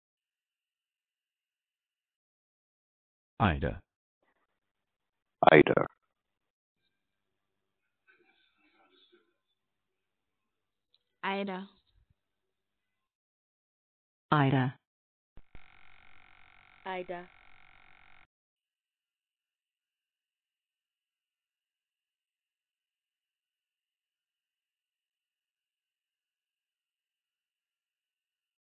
How to Pronounce Idah
This video shows you how to pronounce Idah. Record your own pronunciation, view the origin, meaning, and history of the name Idah